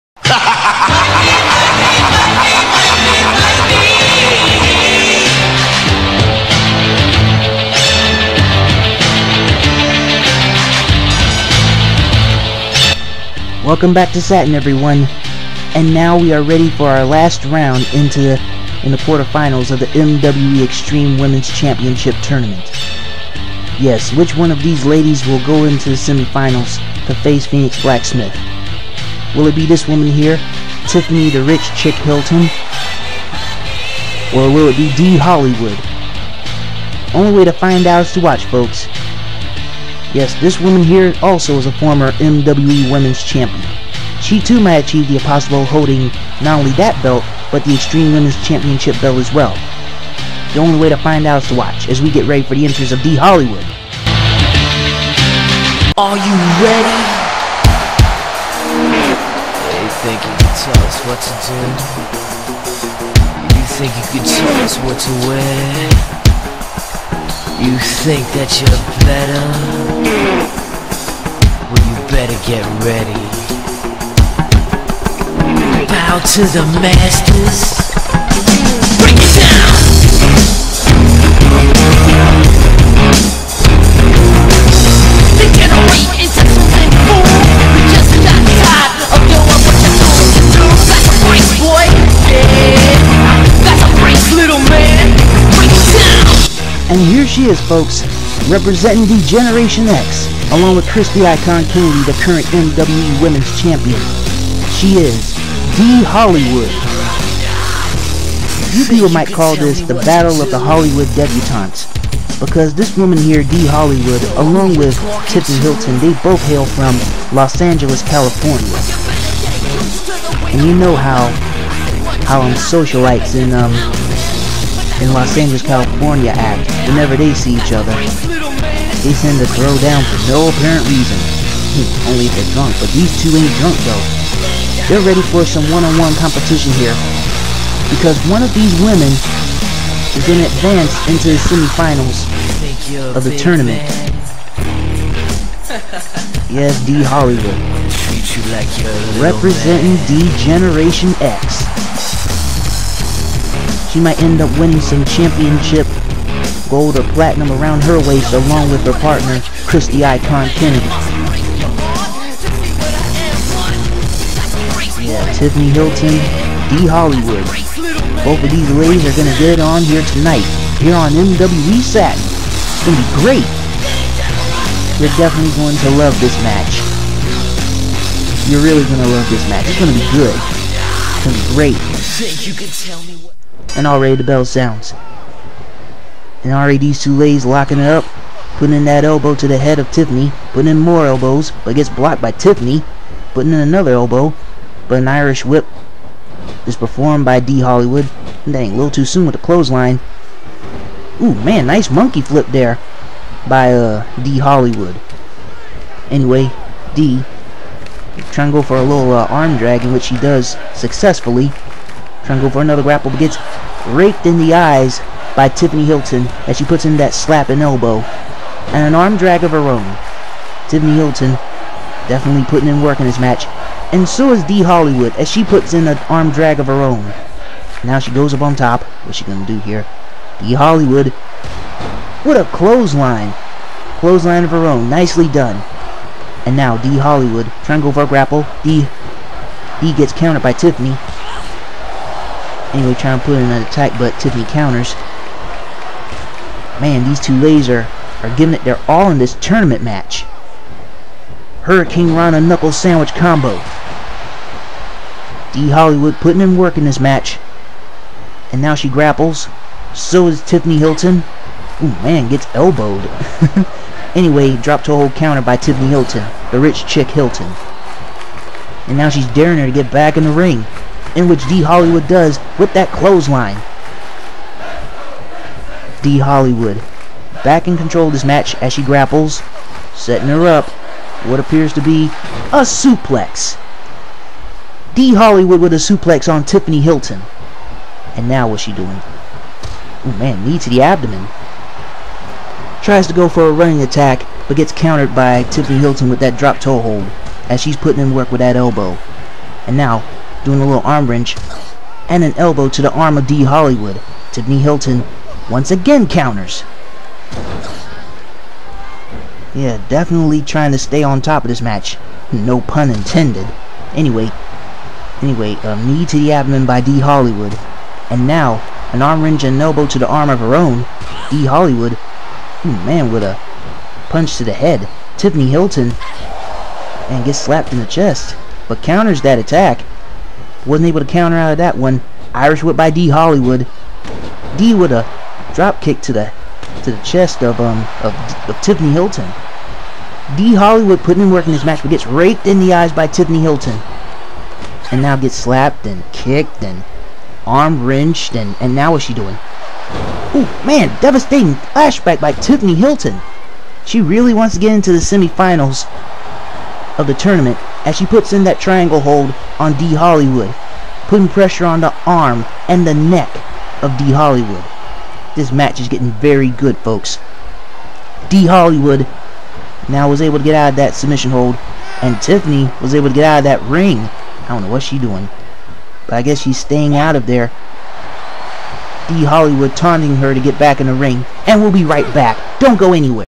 MWE Satin: 2nd Edition(Part 6)
MWE Extreme Women's Championship Tournament-Round 4
Tiffany Hilton vs. Dee Hollywood